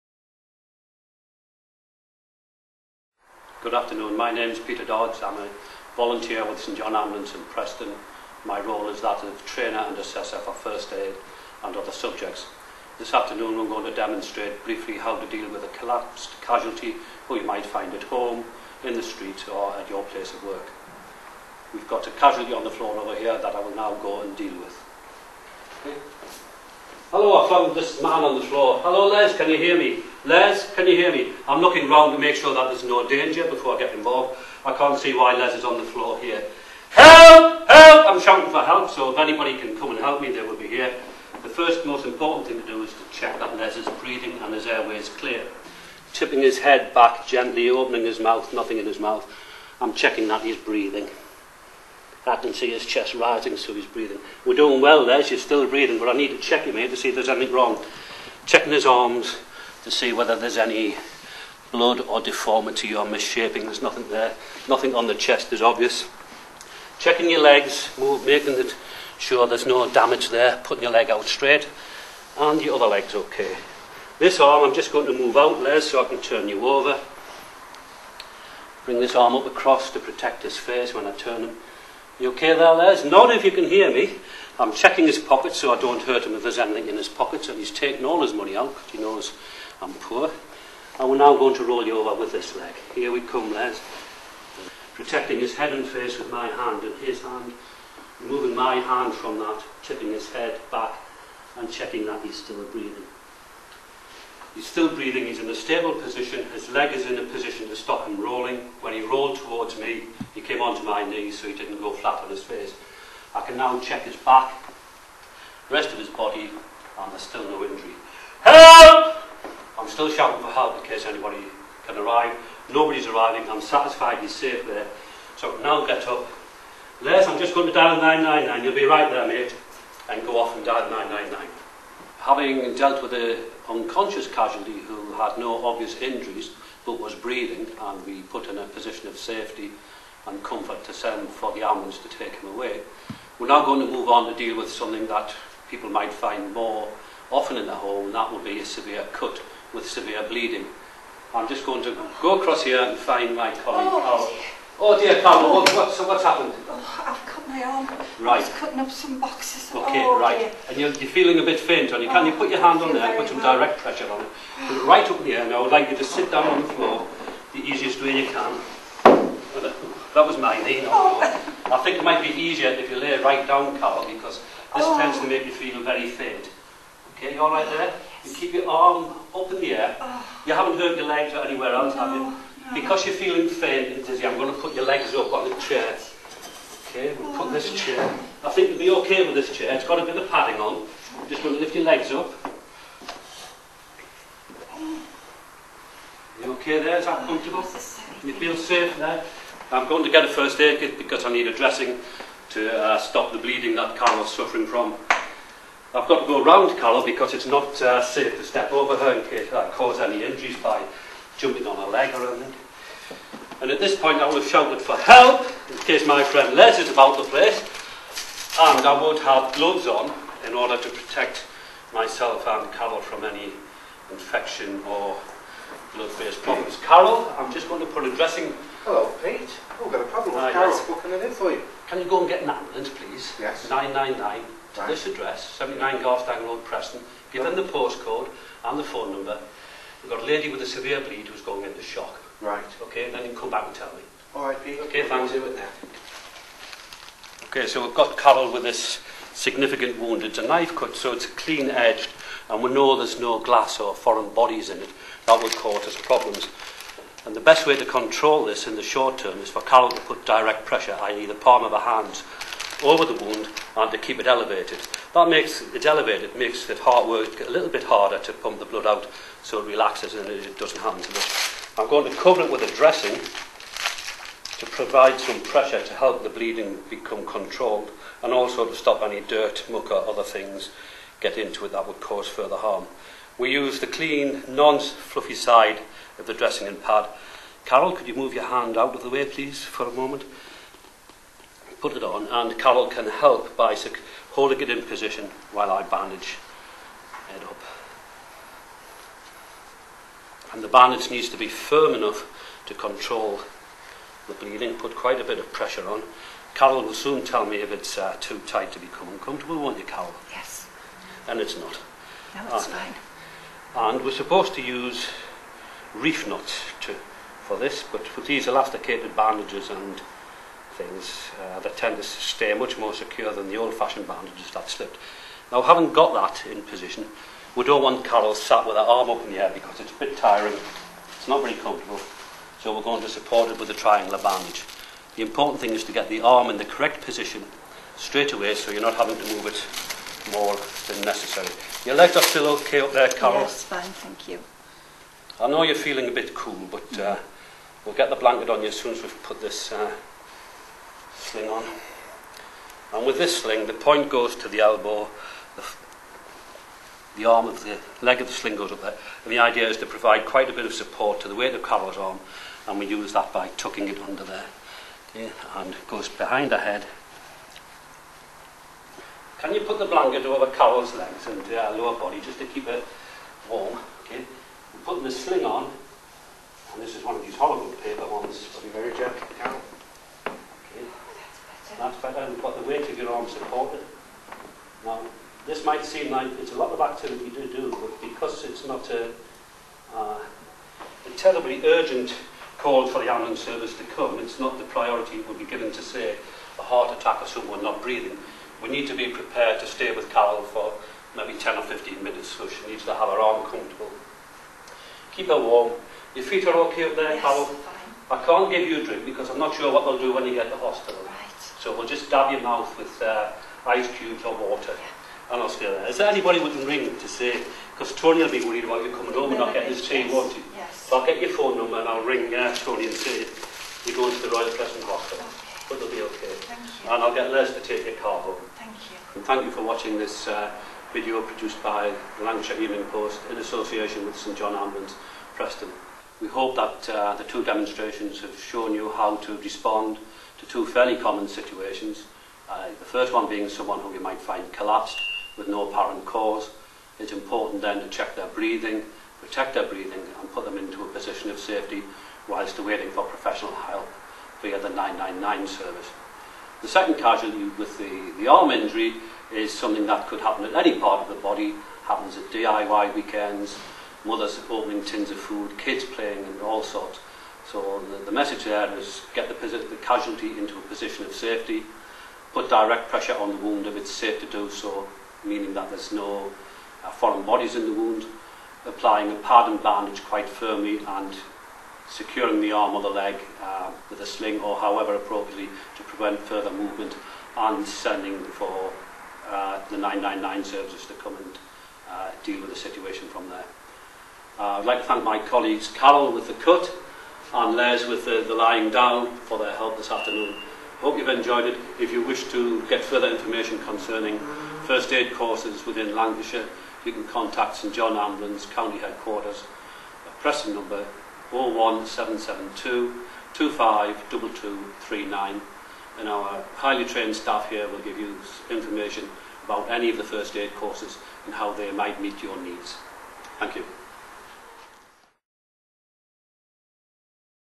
Ask the experts - first aid
Essential first aid with St John Ambulance Trainer Peter Dodds